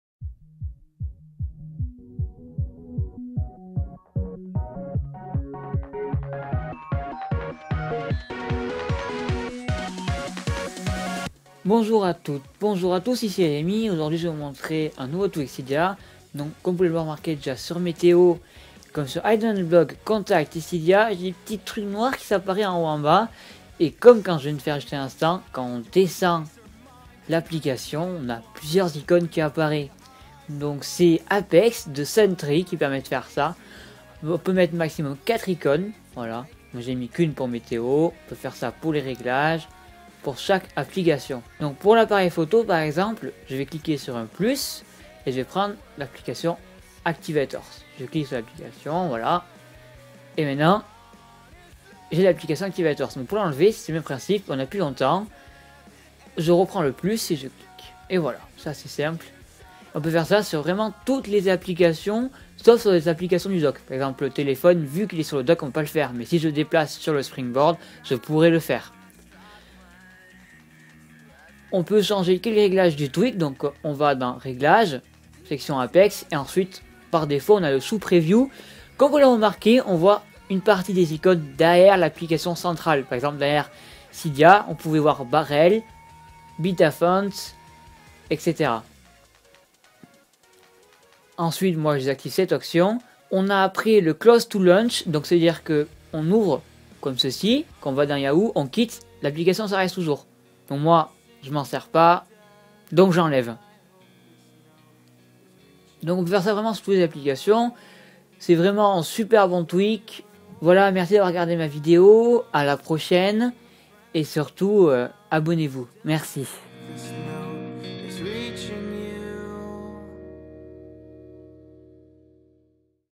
Apex
Voilà un excellent tweak au prix de 1,99$ qui va vous permettre de mettre plusieurs icônes sous une icone.. Bref si vous comprenez pas, regarder la vidéo ;) Ce tweak est pour moi l'un des meilleurs de cette année 2013, révolutionnaire et très peu gourmand en ram, ce tweak est pour moi juste PARFAIT
Pour l'avoir gratuit, il est disponible sur des sources Cydia pas trop légales.. Cherchait sur google, vous trouverait tout ;)